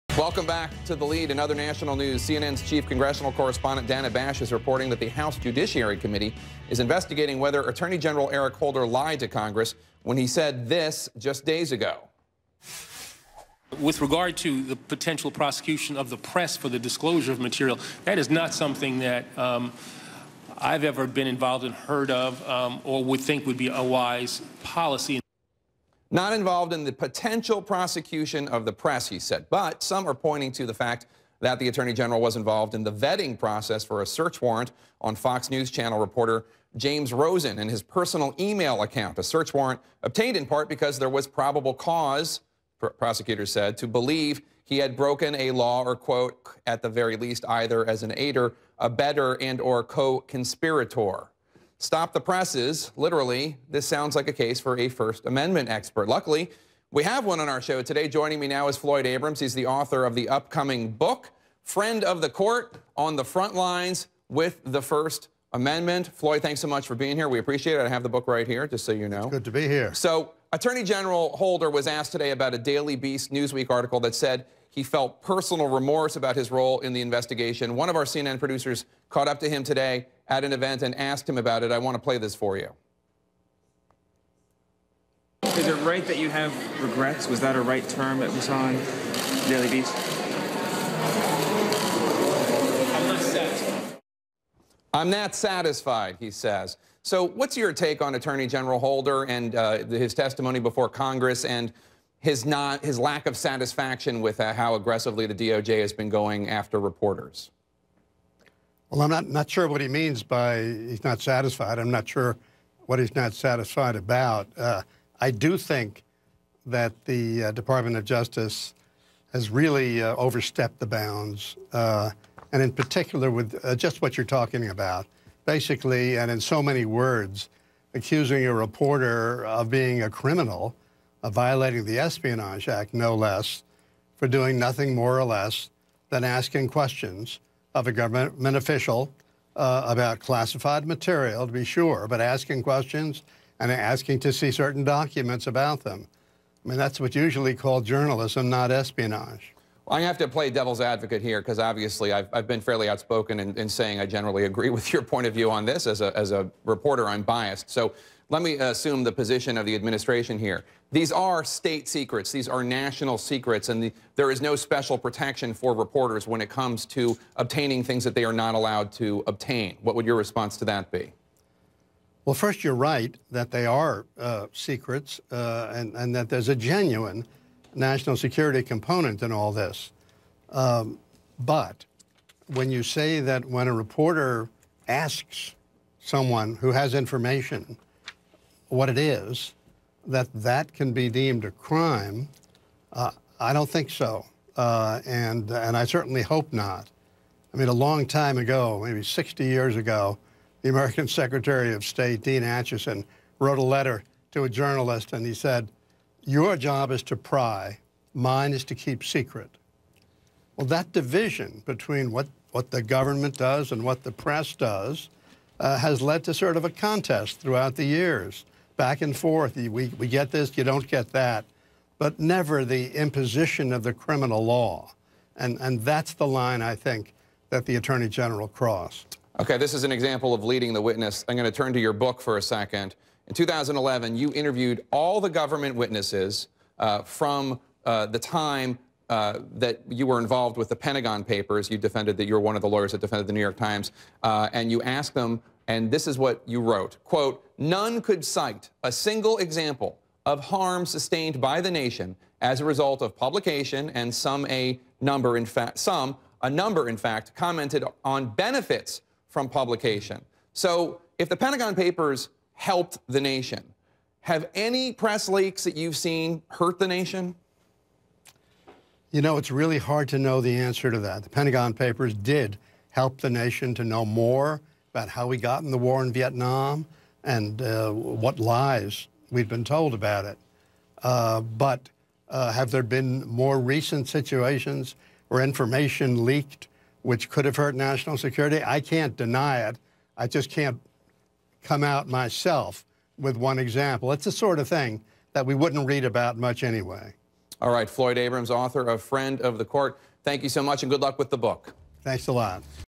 Are classified leaks a problem?
First Amendment expert Floyd Abrams says Justice Department overstepped its bounds in pursuing Fox reporter James Rosen. For more CNN videos, visit our